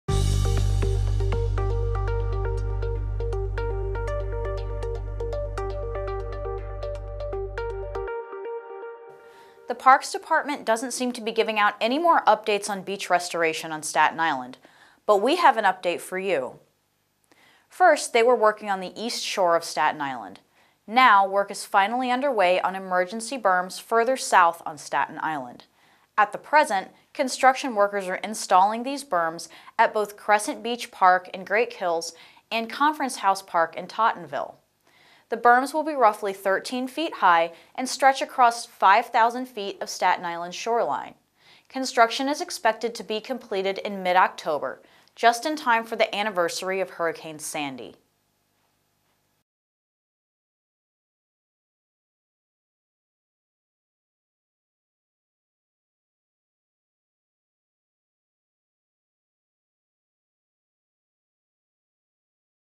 Berms Being Constructed at Staten Island's South Shore Beaches
| Work began a few weeks ago on emergency berm installation for Crescent Beach Park and the beach at Conference House Park.  Will these berms be completed in time for Hurricane Sandy's anniversary?  Watch to find out.